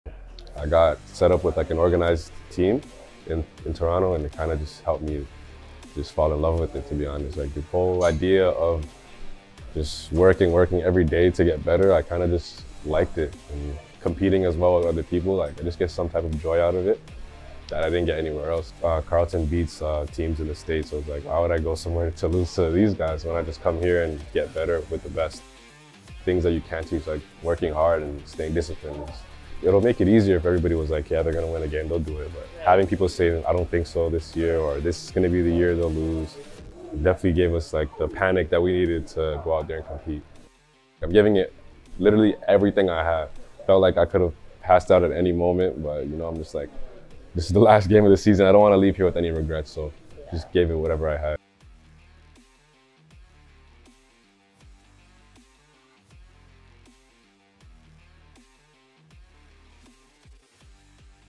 Wazir Latiff, BGInS, Carleton Raven's Men's Basketball
Carleton University Bachelor of Global and International Studies (BGInS) student Wazir Latiff, guard on the Carleton Ravens men’s basketball national championship team this year, with 249 points for the season. He brought not only immense skills, but a sense of energy and joy every time he stepped on the court.